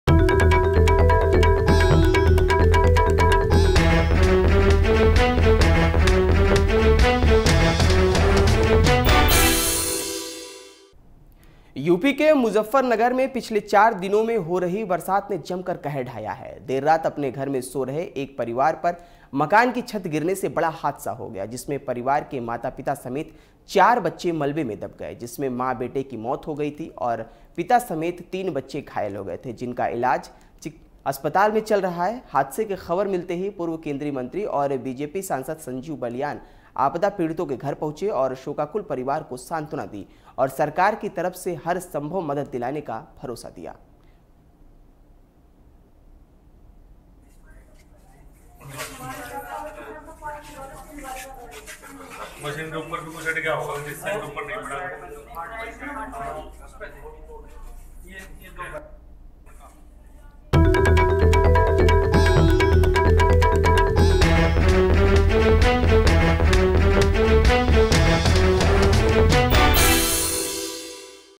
UP के Muzaffarnagar में बरसात ने ली फिर एक जान
यूपी के मुजफ्फरनगर में पिछले 4 दिनों से हो रही बरसात ने जमकर कहर ढाया है। देर रात अपने घर में सो रहे एक परिवार पर मकान की छत गिरने से बड़ा हादसा हो गया।